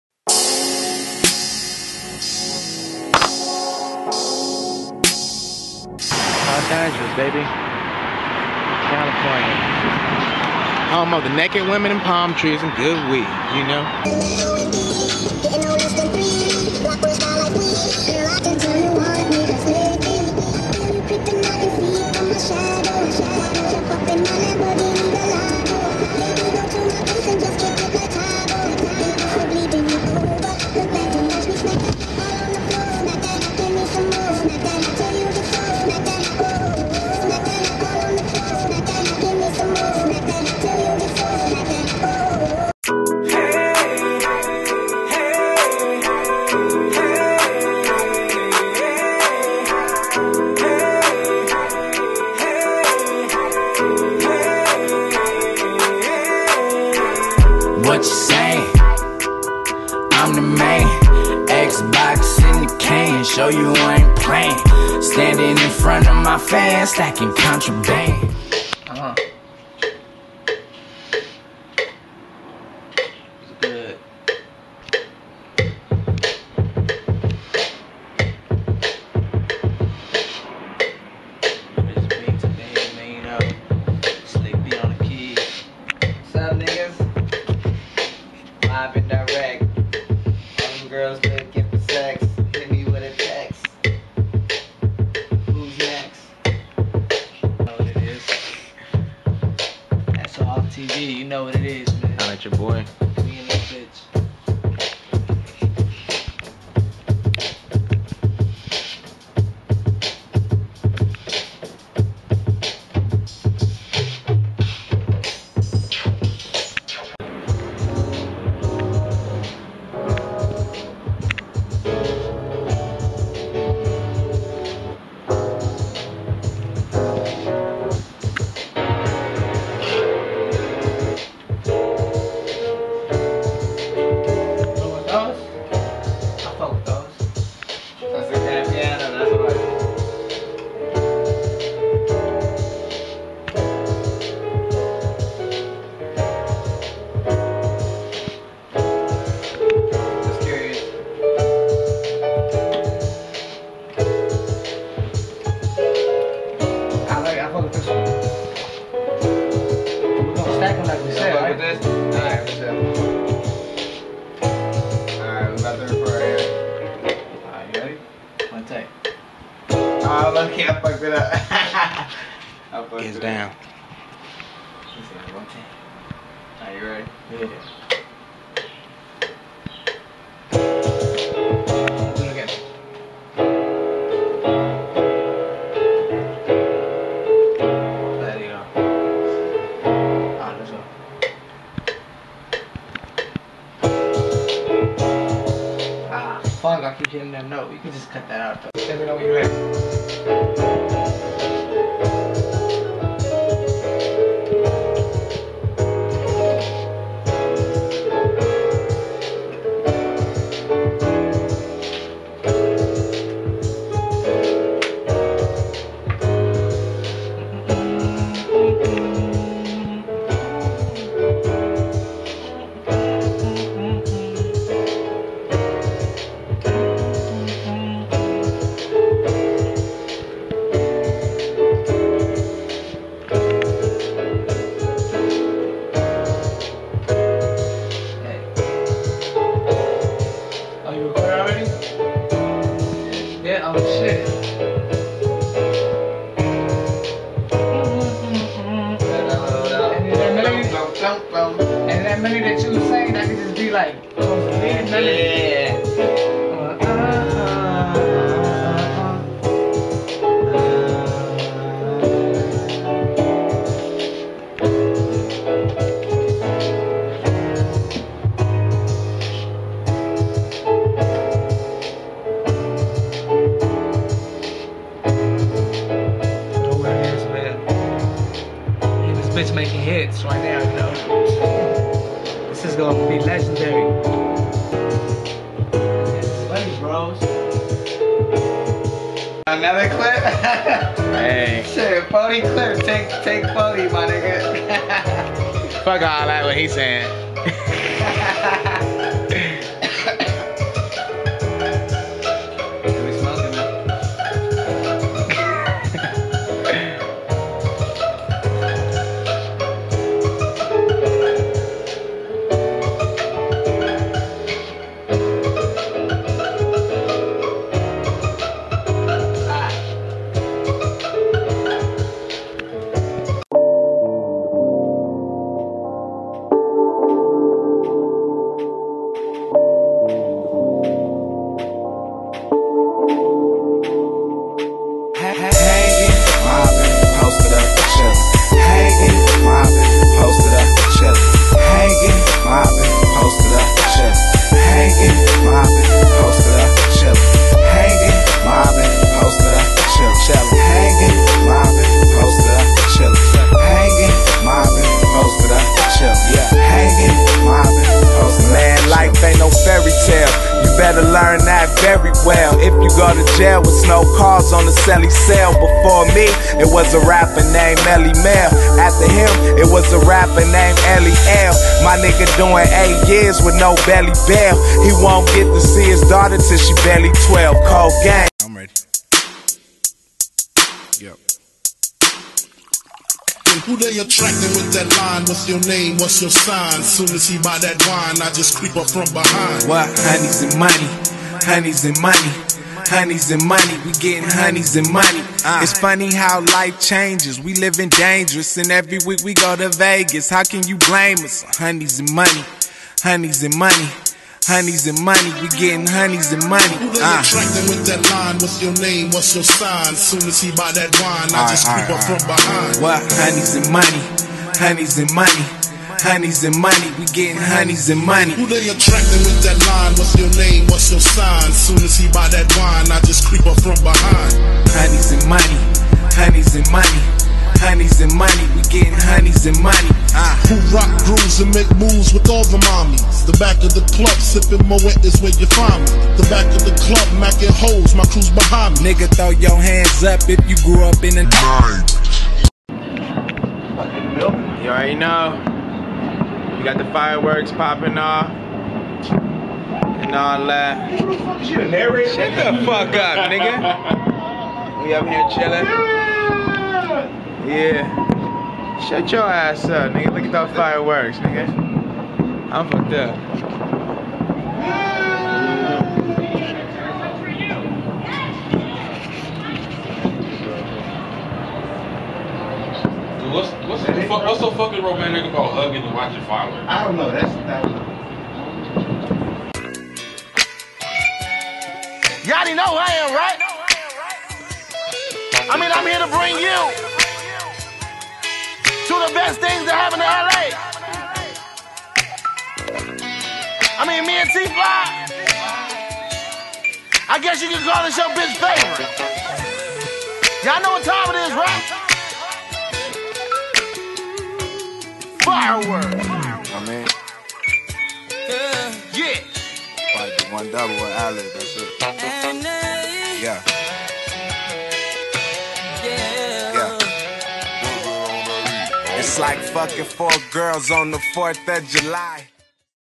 HasselhoffTV in 2012
In 2012, HasselhoffTV goes through cities such as San Pedro, Downtown Los Angeles, Inglewood and etc. Speical guest apperance from SLik d & MrChrisBeatz. Also HasselhoffTV watches FireWorks in Las Vegas.